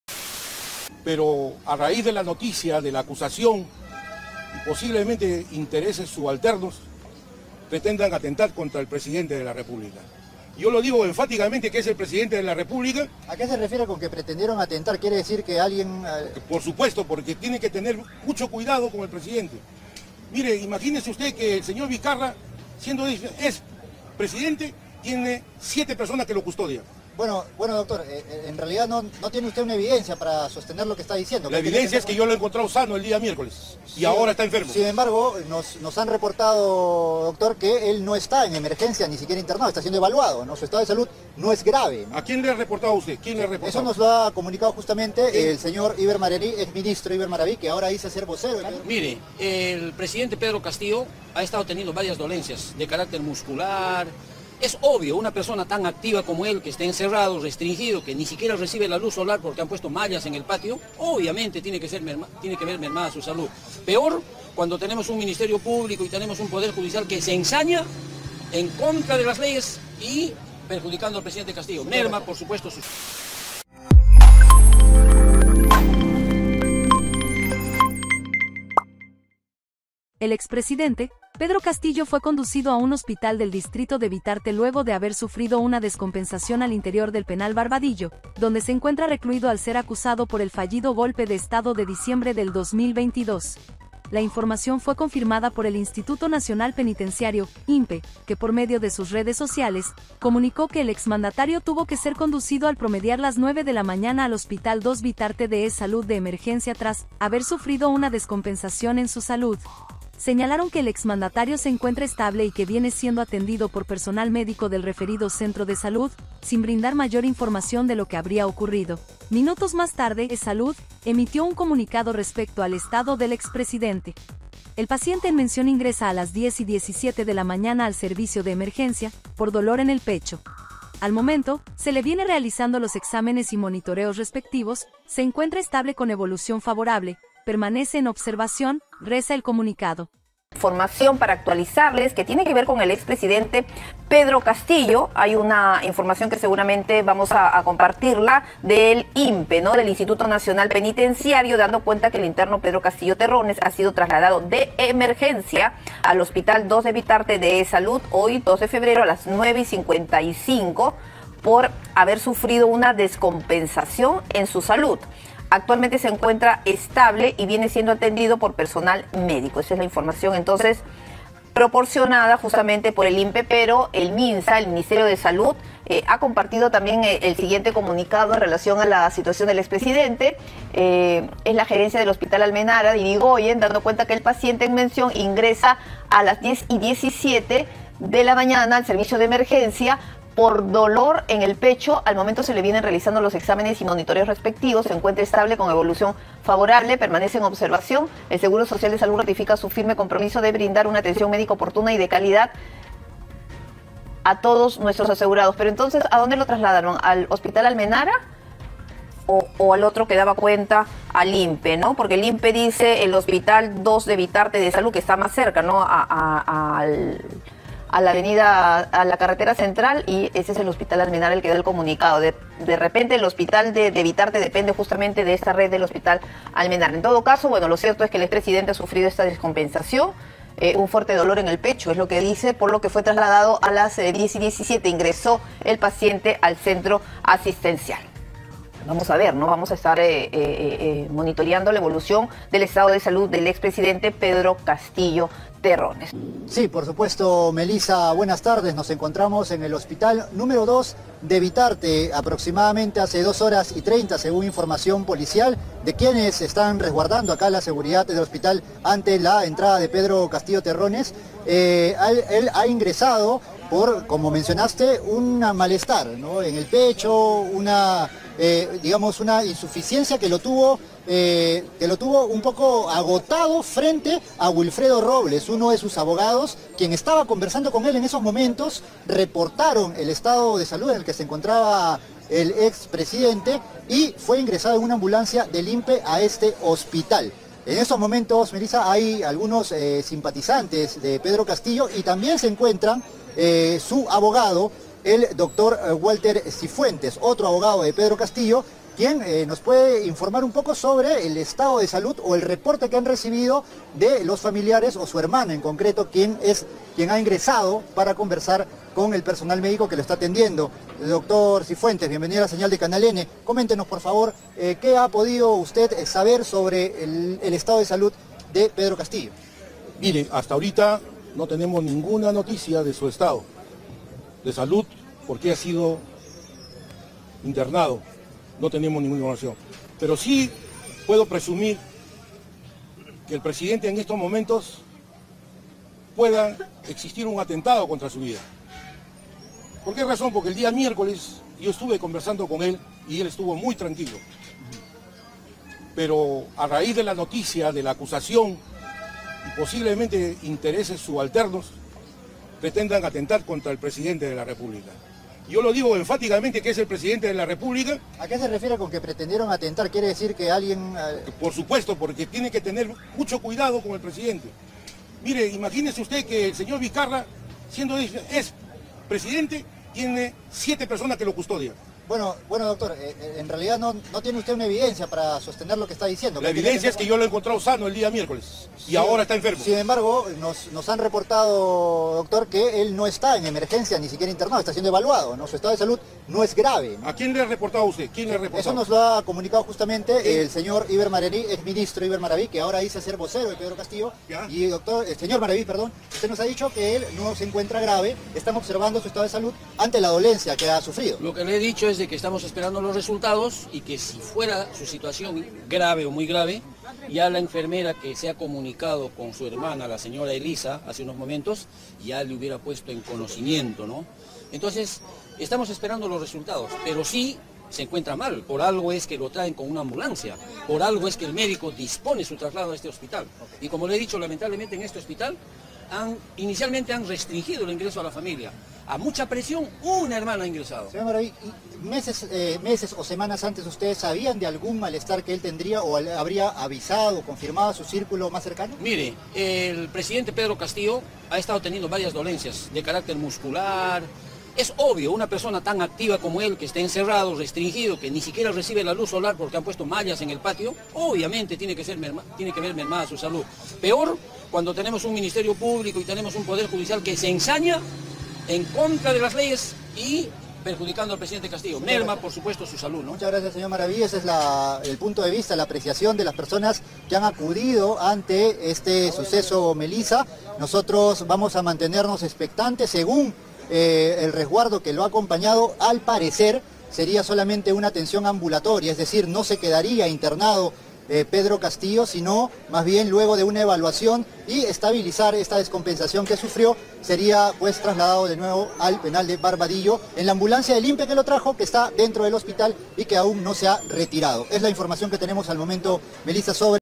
Castillo fue trasladado de Emergencia, y su abogado queda en ridículo x decir q lo querían m4t4r
Pedro Castillo fue trasladado de emergencia a hospital tras sufrir una descompensación
EsSalud señaló que Pedro Castillo permanece en evaluación tras ingresar con un dolor en el pecho al establecimiento de Salud. Su abogado, Wilfredo Robles, dio más detalles al respecto.